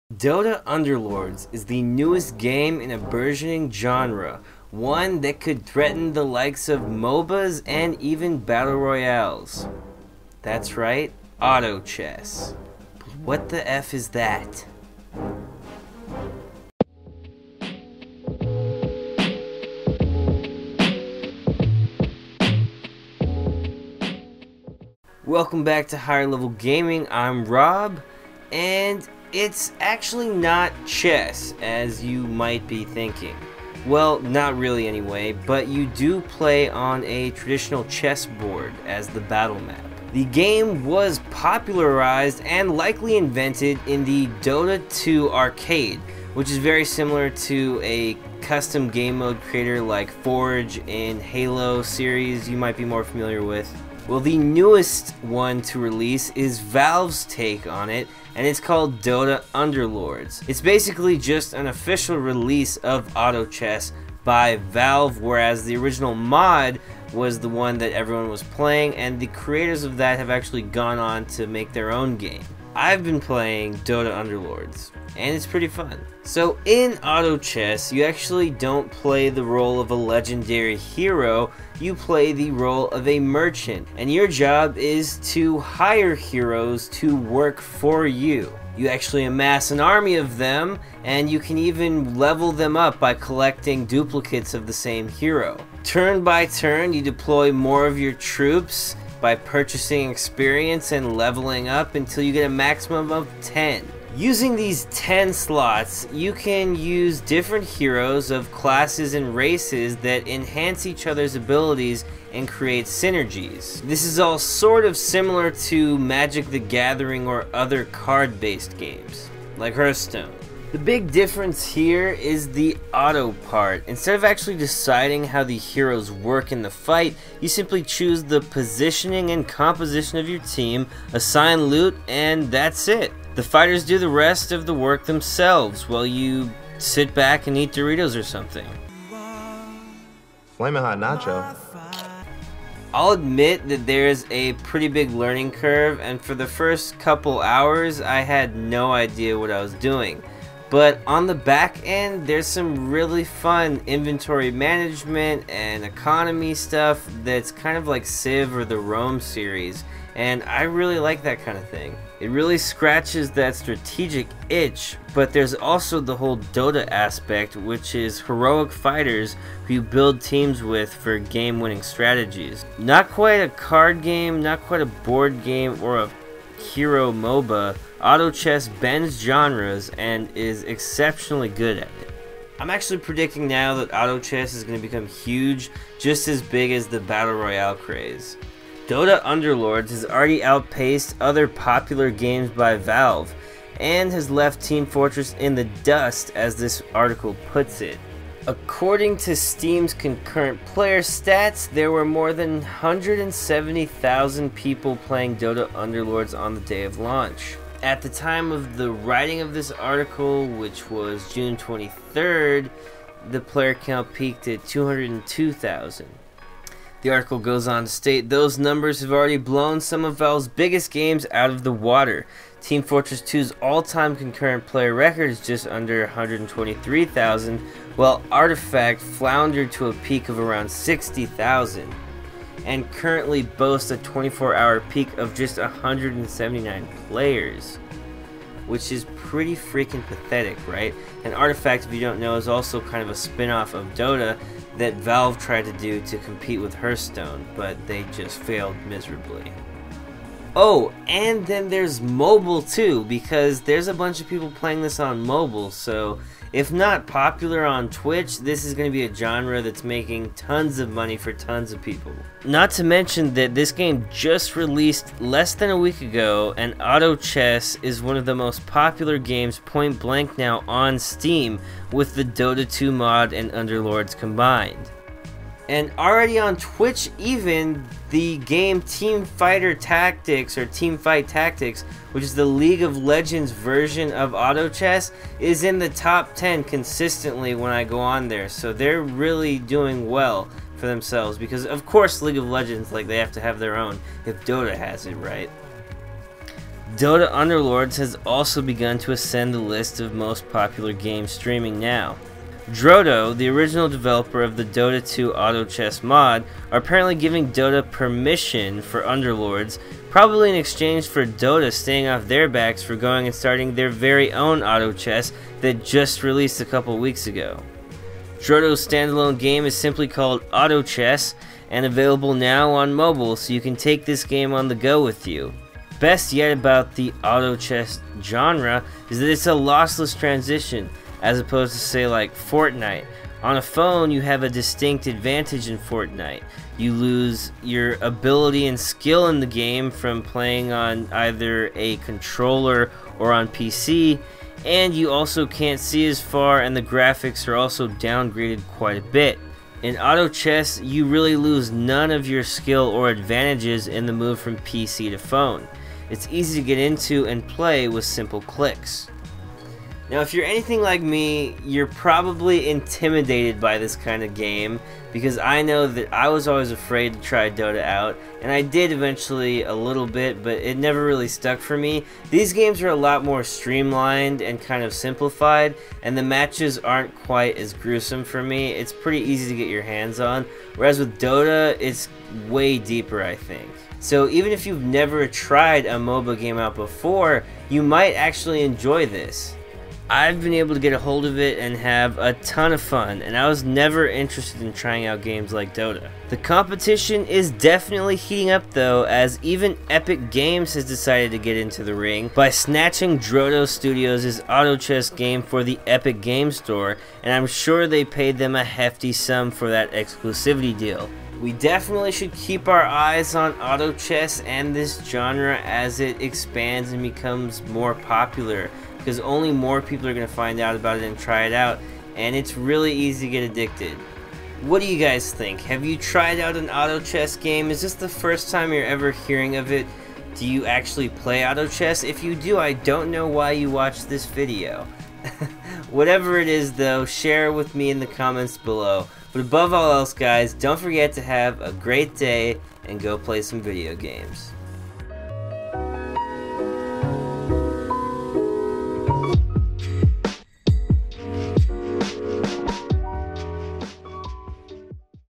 What the F is Auto Chess? Introduction to the genre & mechanics!
Hey there and welcome to HigherLevelGaming!
If you enjoy, please remember to